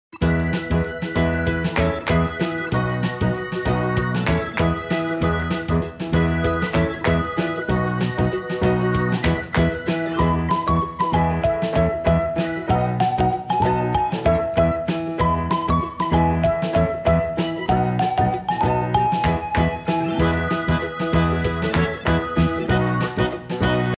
DIY How to Make Doll Dress/ Barbie Dress
DIY How to Make Doll Dress/ Barbie Dress Today I am making a cute little pink dress for a Barbie doll by adjusting the size you can make it for American girl , Monster high,Journey girl, or Ever After dolls. Hey guys please tell me witch DIYs you would like to see me do next.